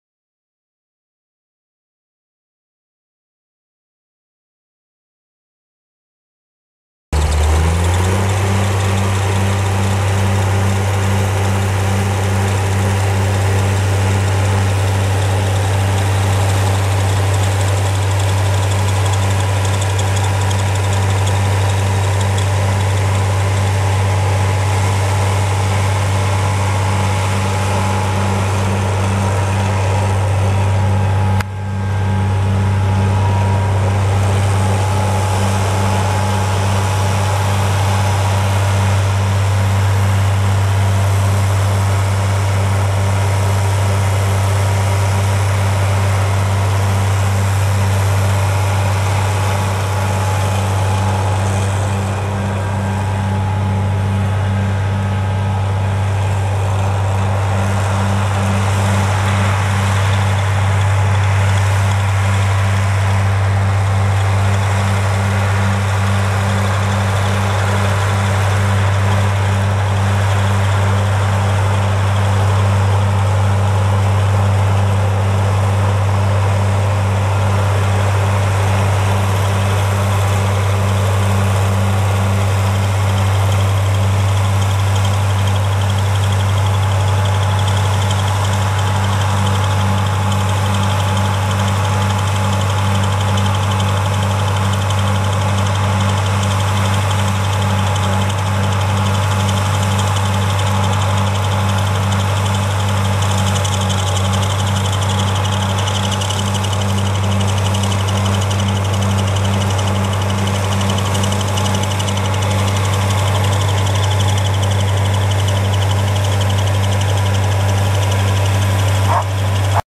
White 2-135 Field Boss tractor for sale | sold at auction December 12, 2012 (New)
SOLD! Ag Equipment Auction This asset was for sale and sold for a contract price of $5830 in an absolute, no-reserve Internet auction by Purple Wave on Dece...

White,2-135,Field,Boss,tractor,for,sale,|,sold,at,auction,December,12,,2012